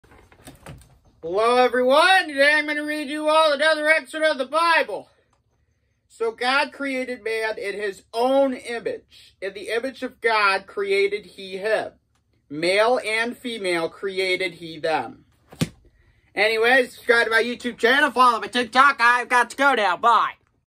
Ethan Huber Bible Study 28
Today, I read you all another excerpt of the Bible.